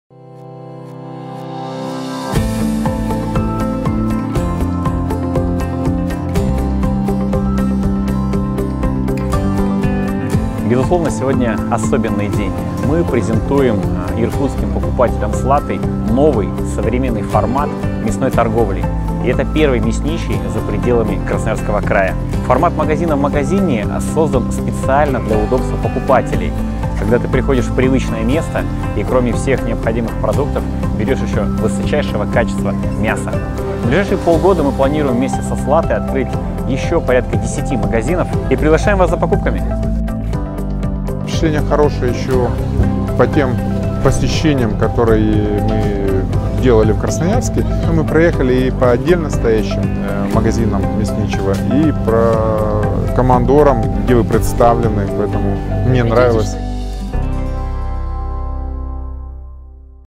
Первый "Мясничий" в Иркутске
Открытие первого магазина "Мясничий" в супермаркете "Слата": Иркутск, 2 ноября 2018 г.